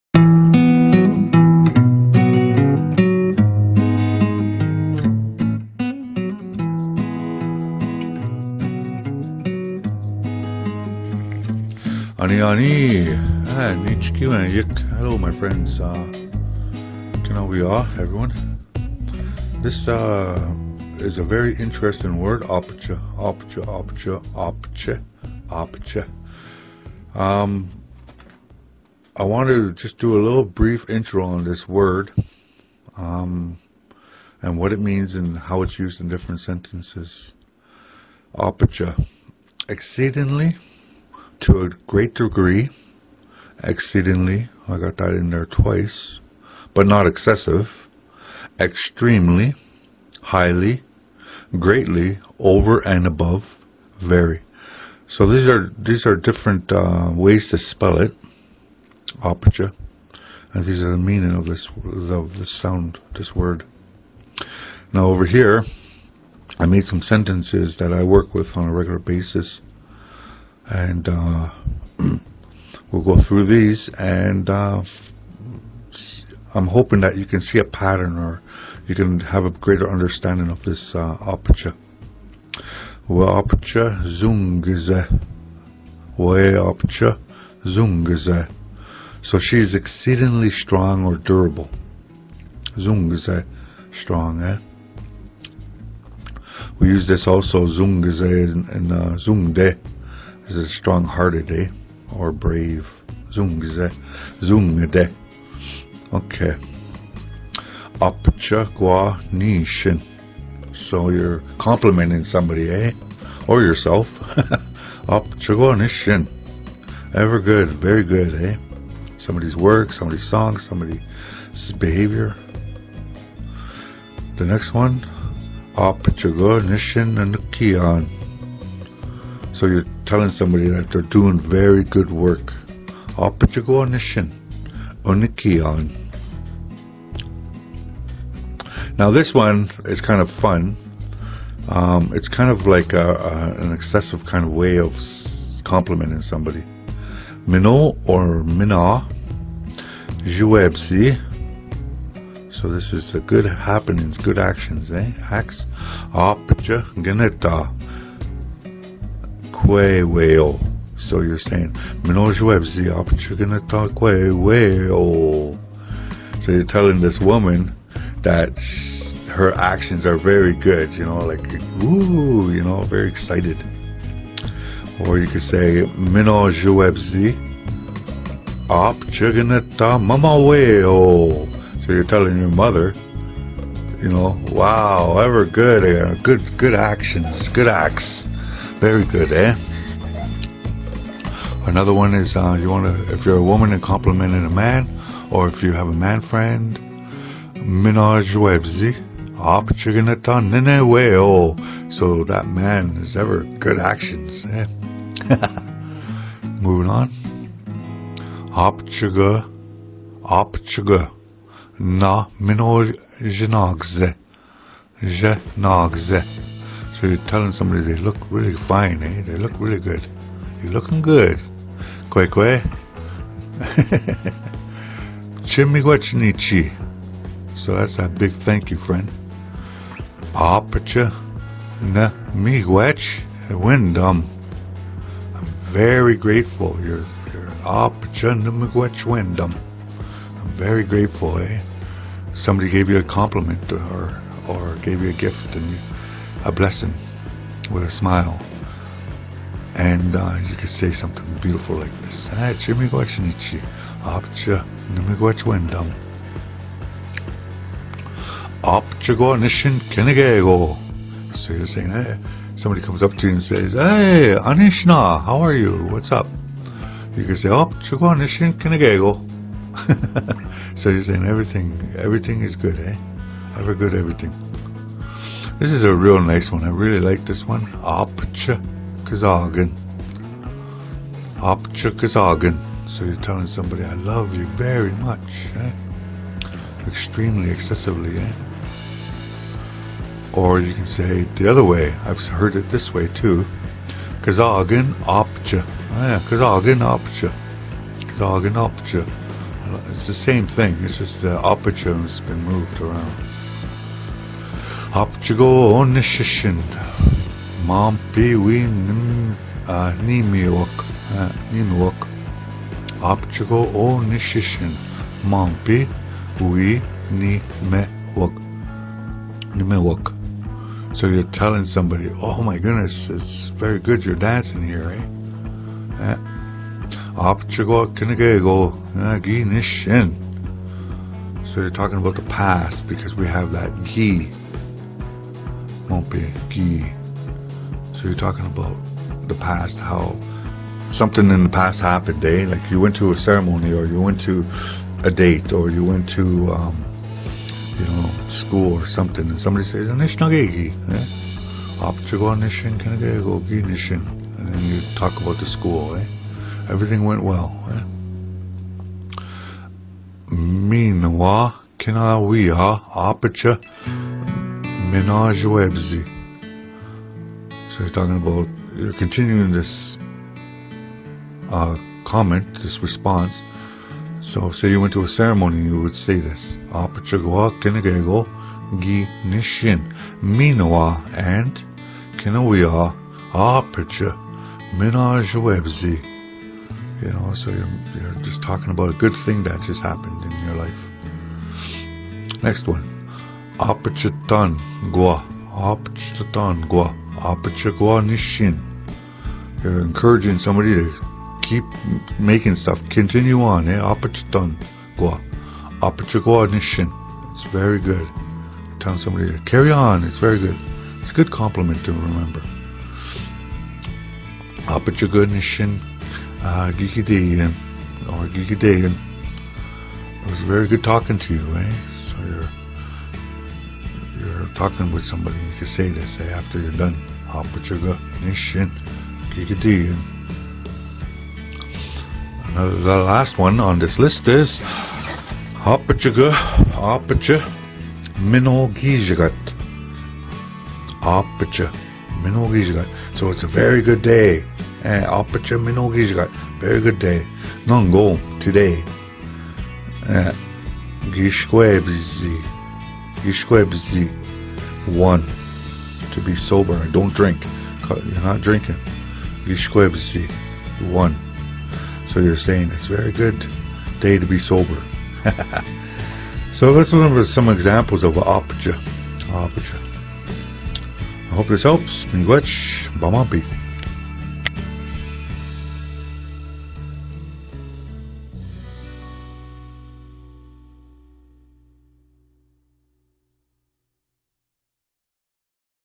"Abiji" word of the day
Abiji is a word that is used alot in speaking...
Adverb (help to modify verbs)

Aabji / aabiji / Aapiji / aupitchi / apitchih
exceedingly , to a great degree, exceedingly but not excessive,
extremely, highly , greatly , over and above , very,

W’aupitchi zoongizih…….s/he is exceedingly durable / strong

Aapiji (gwa) Niishin……….Ever /very good

Aapiji gu Nishin enNokiiyan………..Your doing Ever/ very good work

Mina (o) Zhiwebzi Aabiji Gnita kwe way yo……Good happenings/acts Very much Woman
                                                       mama way yo
                                                       nini way yo

Aapiji go naa mno zhinaag-zi……you looking really good

Chi miigwetch niiji , Aapiji ni’miigwetchiwendam ….big thank you my friend , I am very grateful

Aapiji gwa niishin  kina gegoo ………..ever good everything

Aapiji Kzaagin ….love you very much
G’zaagin aapiji …I love you very much

Aapiji go Onizhishin maanpii wii-niimiwag ….It is very good your are dancing here

Aapiji gwa kina gego , gii niishin …everything went so well (so good)
miinawa kina wiya aapichi minazhawebzi……and everyone there was so kind

Aabijitaan gwa , Aabji gwa niishin ……..continue on , it is very good

Aapiji gu Niishn giigideyin……it was very good talking to you

Aapiji mno giizhigad nongoom  …it is a good day today to be sober
giiwashkwebiizii waan